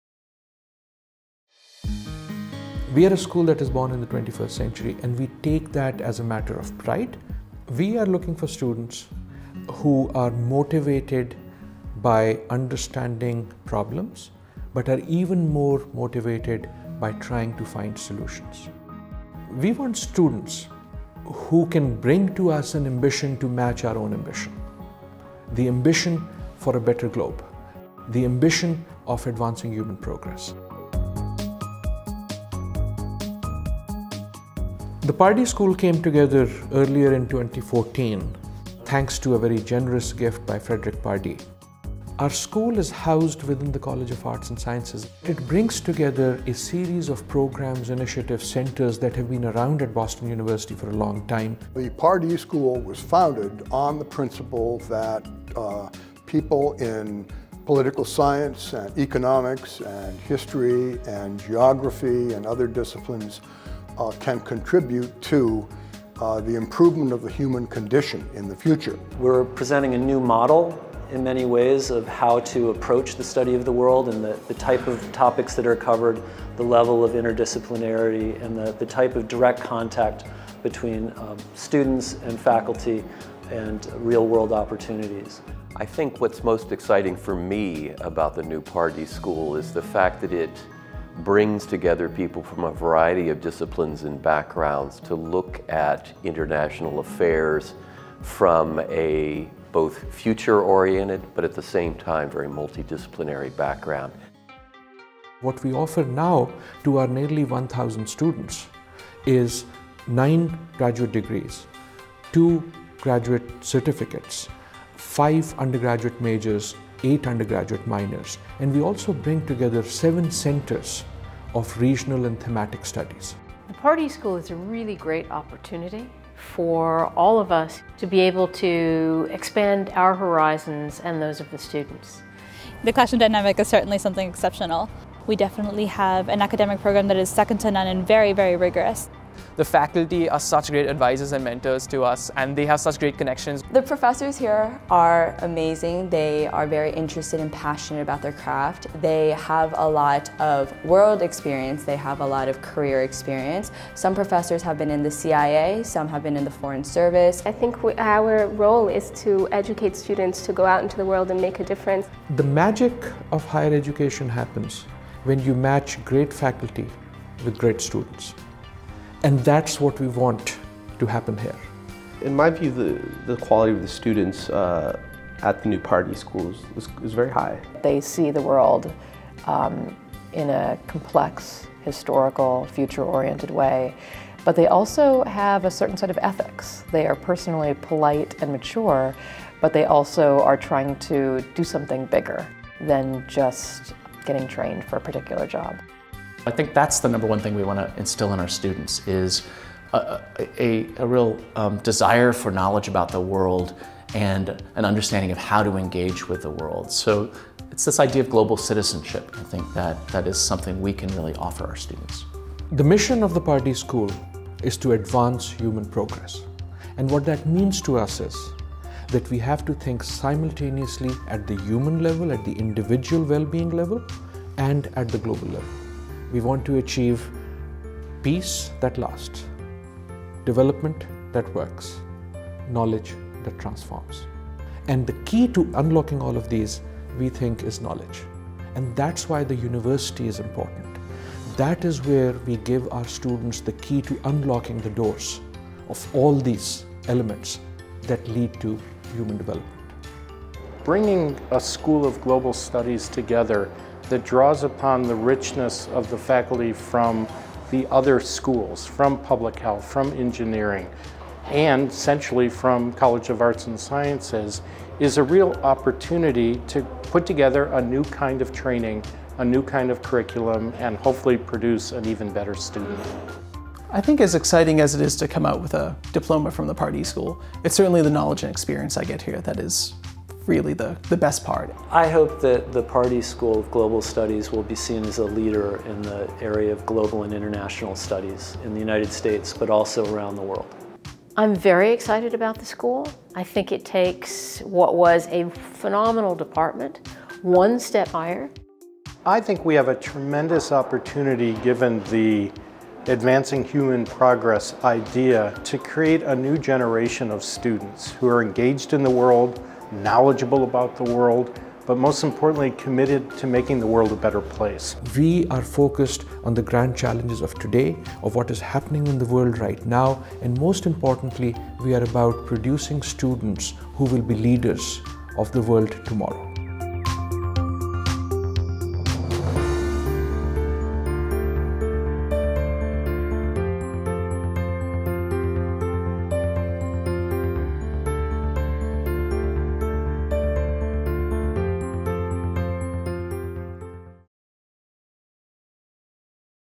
Welcome to the Frederick S. Pardee School of Global Studies at Boston University
Peace that Lasts. Development that Works. Knowledge that Transforms.

The Pardee School of Global Studies was established in 2014 with the mandate of advancing human progress. Building on Boston University’s strong foundation in International Relations, the Pardee School is home to five undergraduate majors, nine MA programs, and an ever growing list of regional and thematic studies centers to provide cutting edge, multidisciplinary scholarship and research.